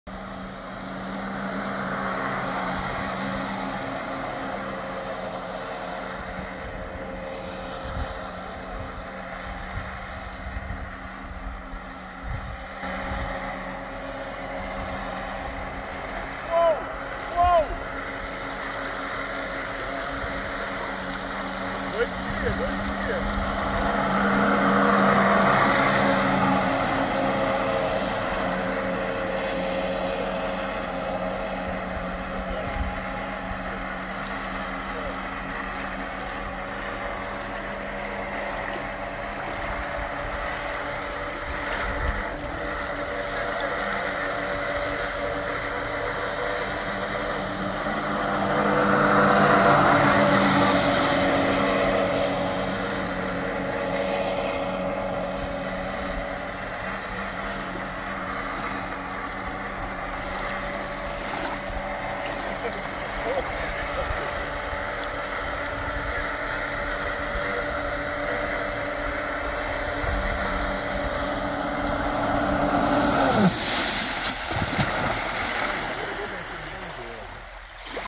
Self Build Electric Surfboard, first time use
Will soon make a short video on how I build it with all the parts i used.